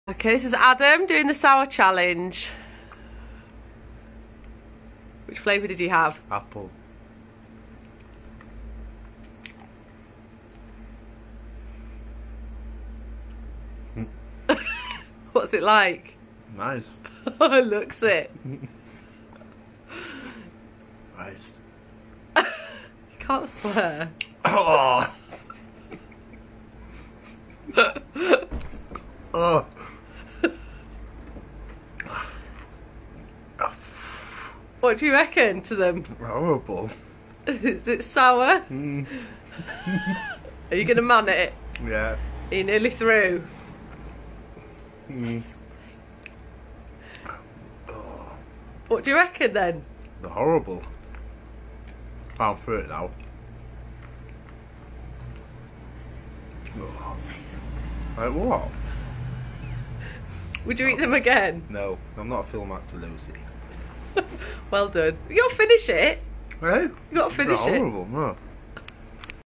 Adam's Bah Humbugs Mega Sour Challenge
Here's my little brother attempting the Mega Sour Challenge... doesn't manage to eat it all and boy were his eyes watering! If you want to have a try, to buy your sours!